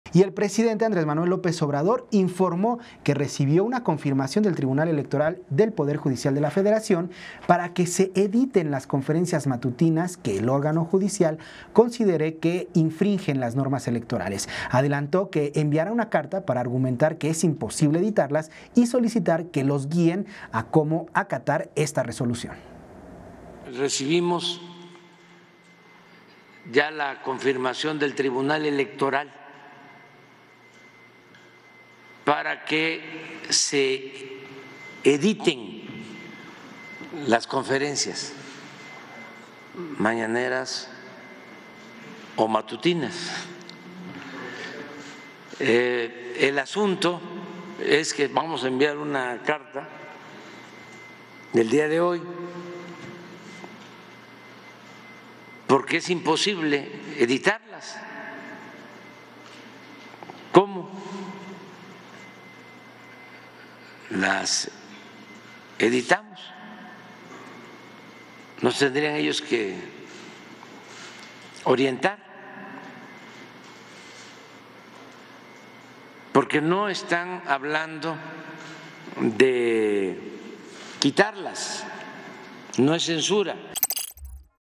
AMLO enviará carta al TEPJF para pedir orientación para la edición de sus mañaneras
SPRInforma | El presidente López Obrador declaró que enviará una carta al TEPJF para solicitar orientación para editar las ConferenciasMatutinas que el órgano judicial consideró que infringen las normas electorales.